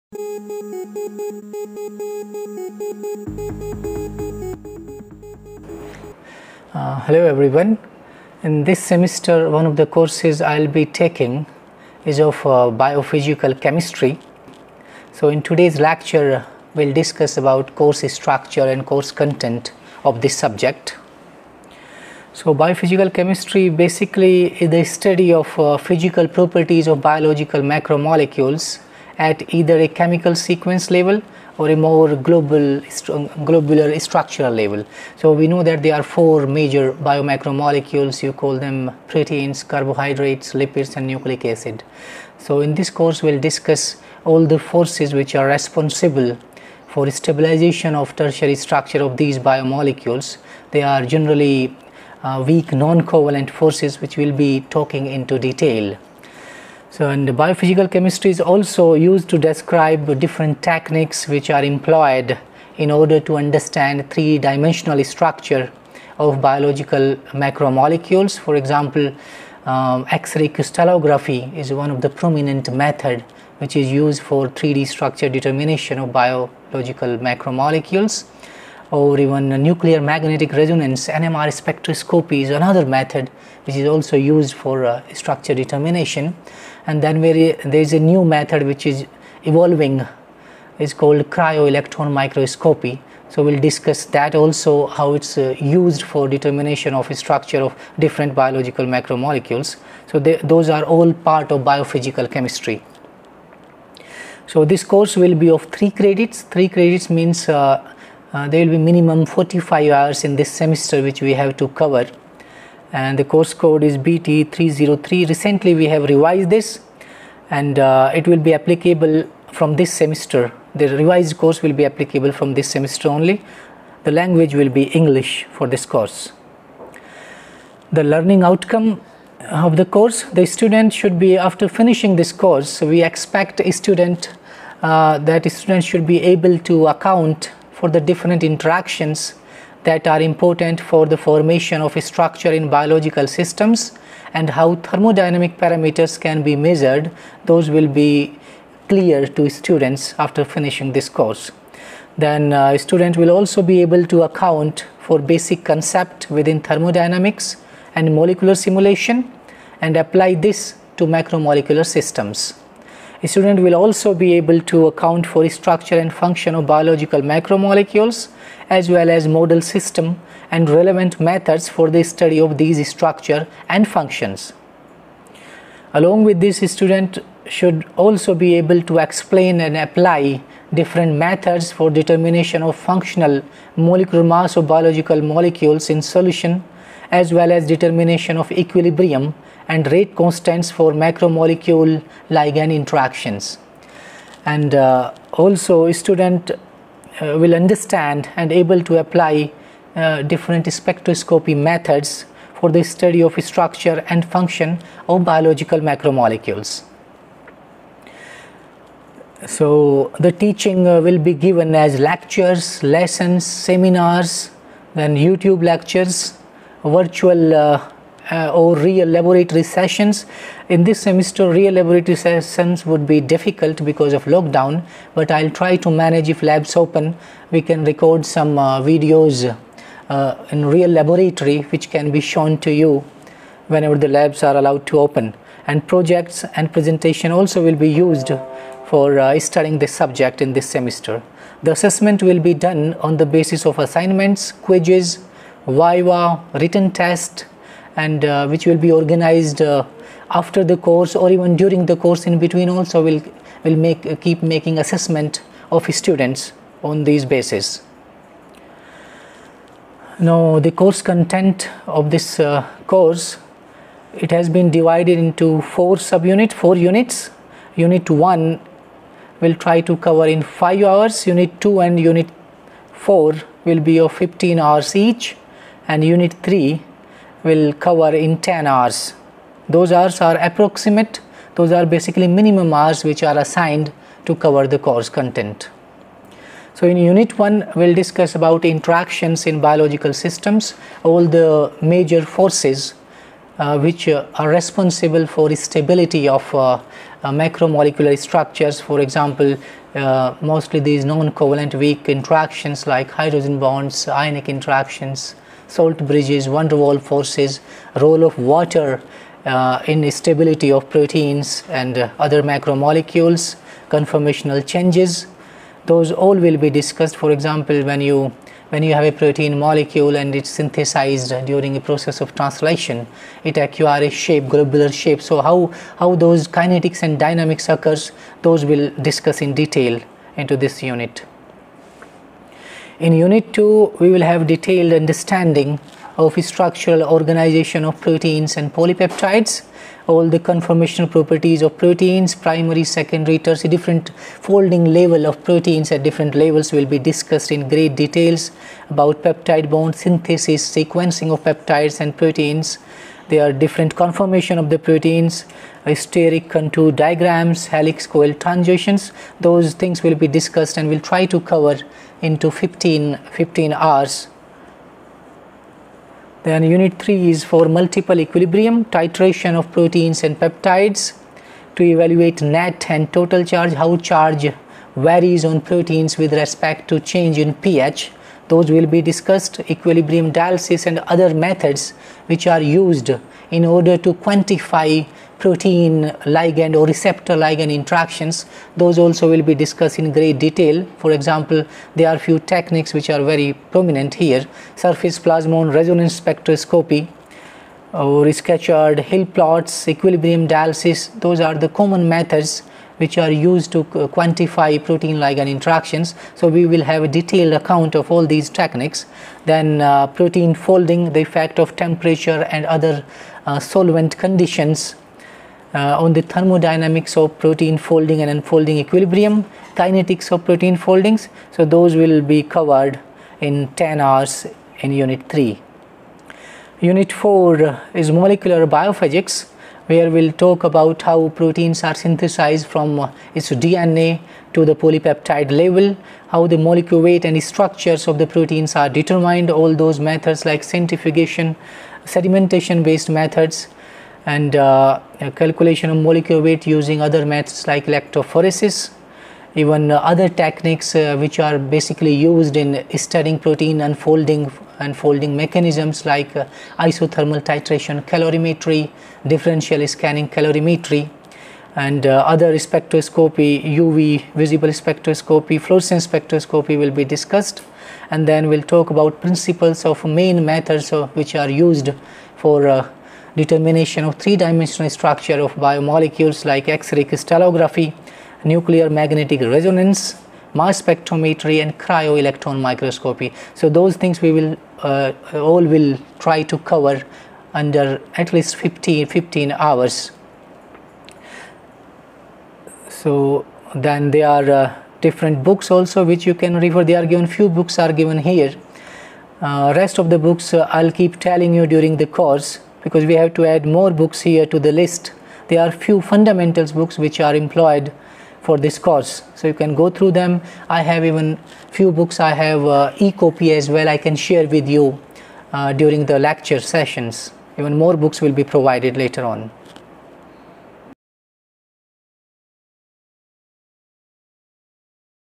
Biophysical Chemistry Syllabus | Course Content | Dr. Nagendra Singh | PENS#29
ideo Lecture given by Dr. Nagendra Singh, Assistant Professor, Gautam Buddha University, Greater Noida, INDIA
ugc net preparation | ugc net preparation online | ugc net preparation tips | UGC NET Life Sciences
 Ionization of Amino Acids and Concept of pI/pKa

Biophysical chemistry is a physical science that uses the concepts of physics and physical chemistry for the study of biological systems. The most common feature of the research in this subject is to seek explanation of the various phenomena in biological systems in terms of either the molecules that make up the system or the supra-molecular structure of these systems. Advanced techniques to determine 3-d structure of proteins such as x ray crystallography, NMR and cryo electron microscopy are also part of biophysical chemistry syllabus.